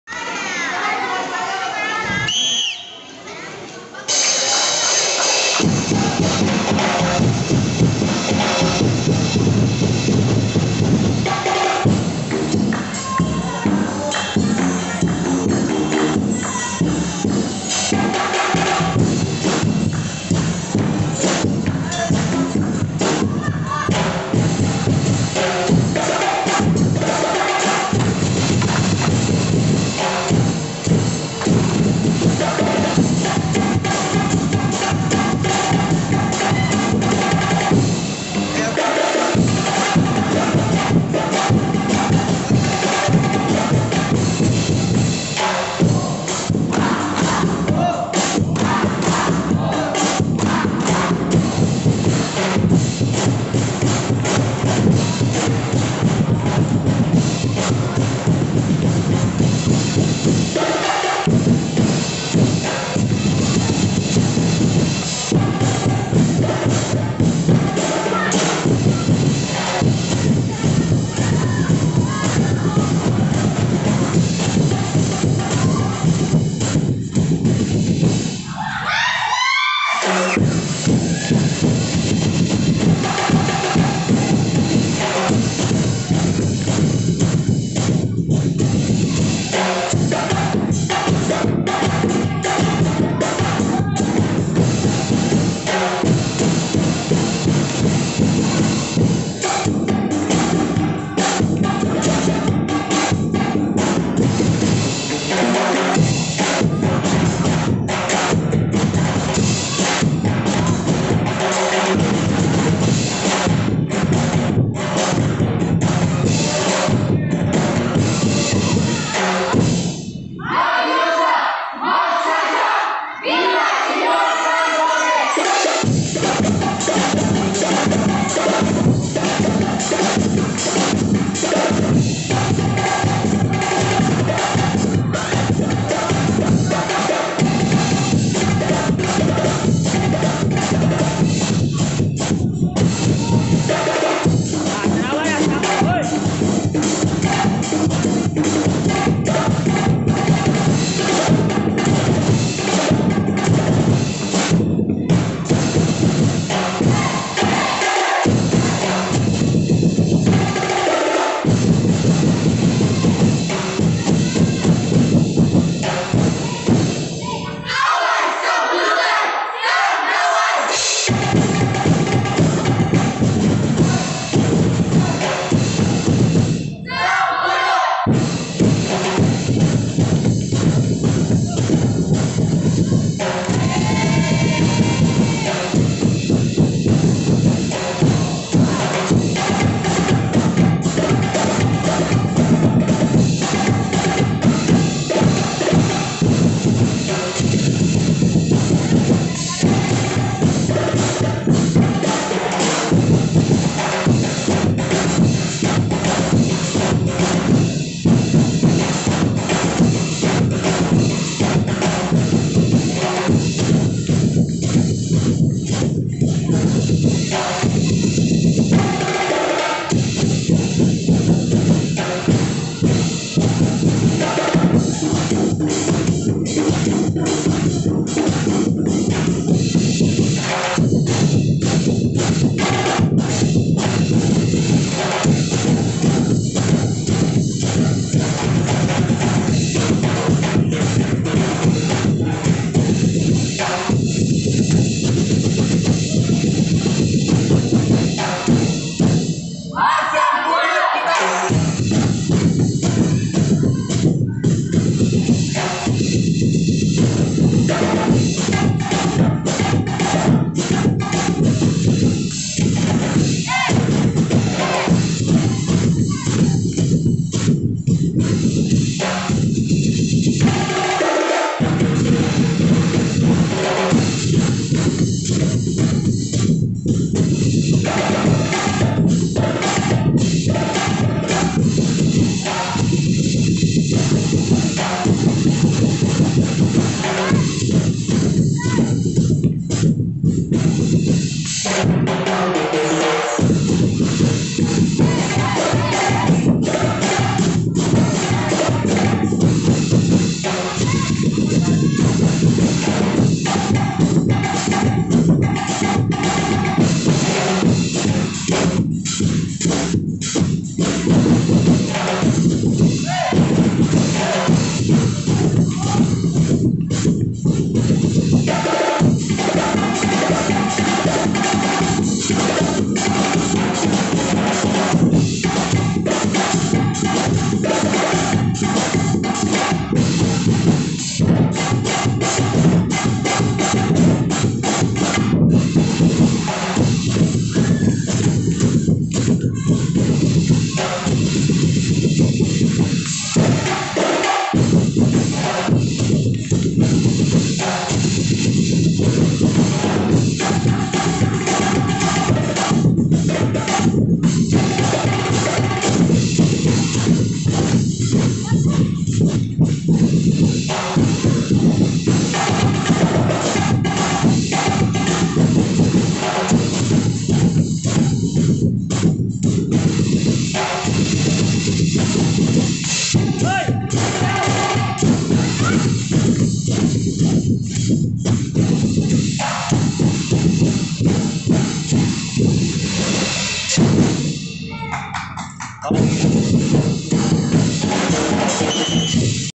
Sambunot Festival 2022 Rehearsal Barangay Candau-ay Purok Everlasting Dumaguete City Negros Oriental
Sambunot Festival 2022
Rehearsal
Barangay Candau-ay
Purok Everlasting
Dumaguete City
Negros Oriental